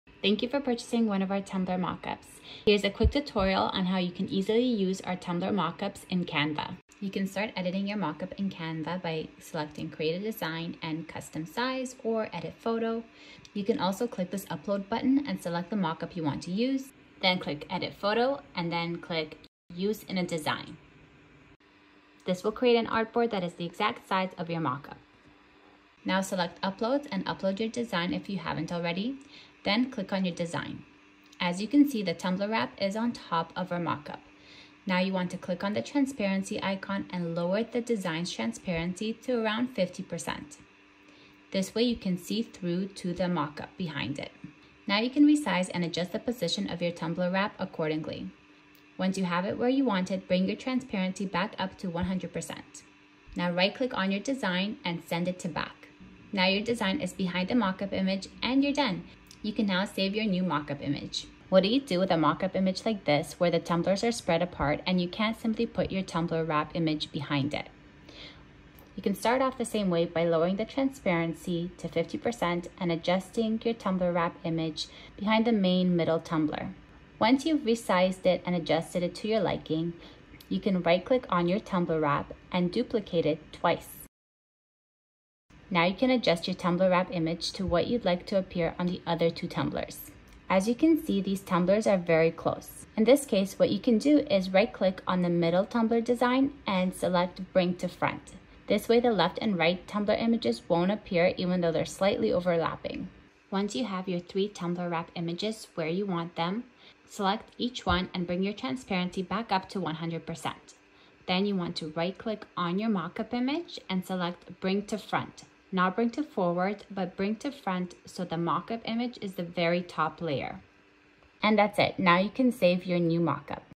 How To Insert Your Tumbler Design to Mock Up in Canva
There are many ways you can use my mock ups, and Canva is one of the simplest. This is a brief tutorial on the steps to insert your tumbler wrap into one of my tumbler mock ups.